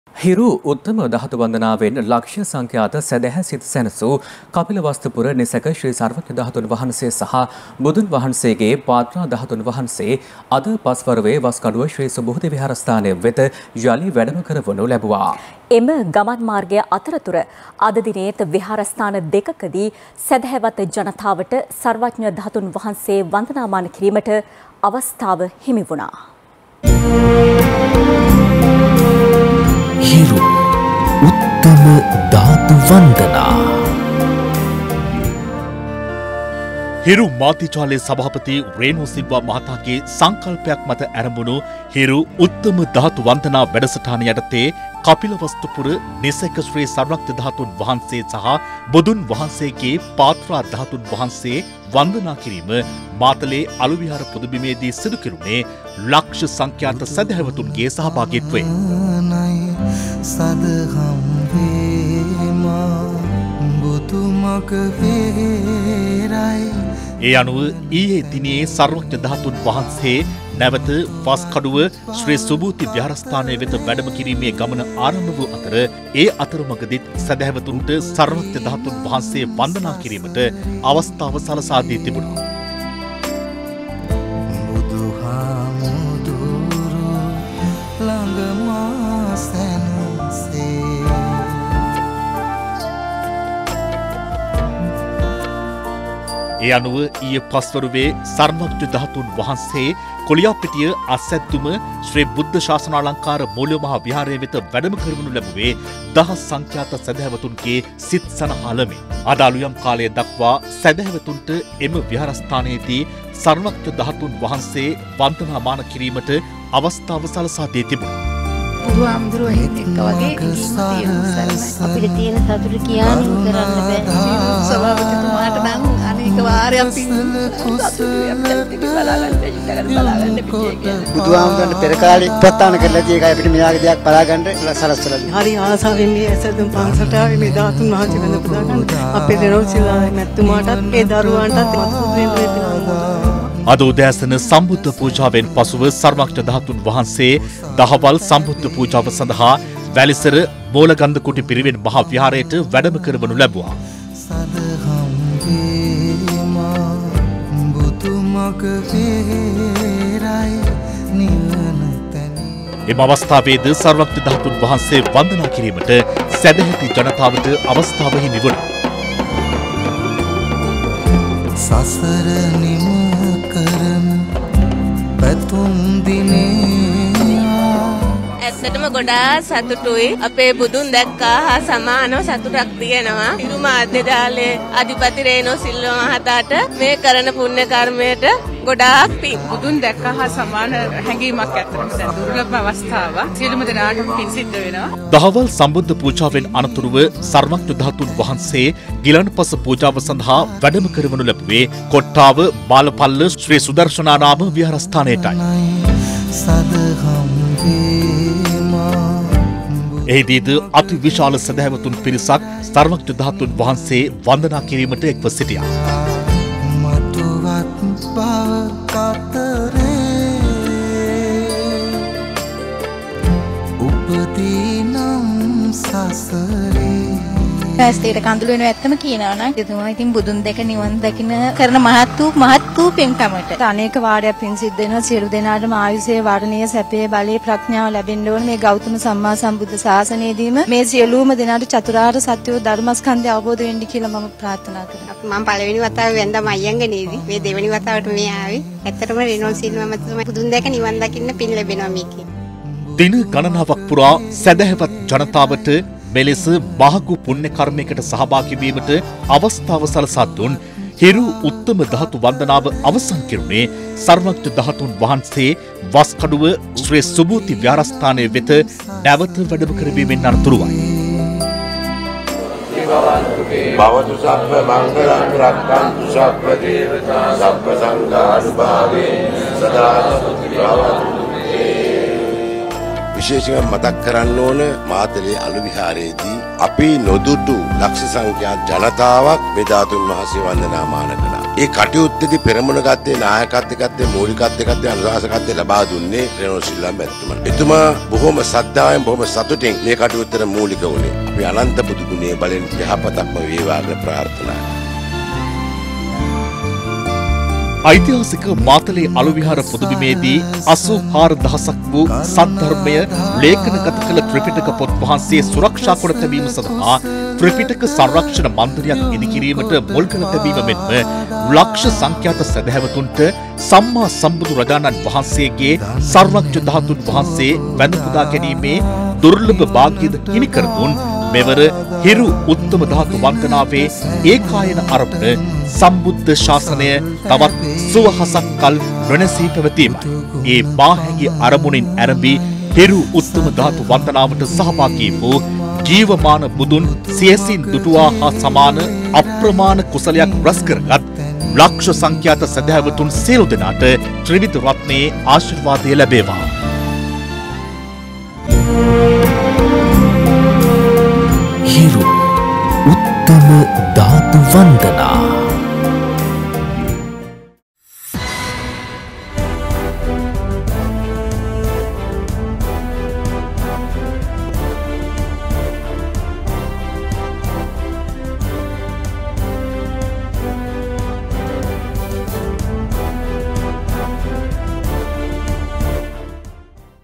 ලක්ෂ සංඛ්‍යාත සැදැහැ සිත් සනහාලන කපිලවස්තුපුර නිසැක ශ්‍රී සර්වඥ ධාතුන් වහන්සේ වස්කඩුව වෙත වැඩම කෙරේ
ලක්ෂ සංඛ්‍යාත සැදැහැ සිත් සනහාලන කපිලවස්තුපුර නිසැක ශ්‍රී සර්වඥ ධාතුන් වහන්සේ  වස්කඩුව වෙත වැඩම කෙරේ - Hiru News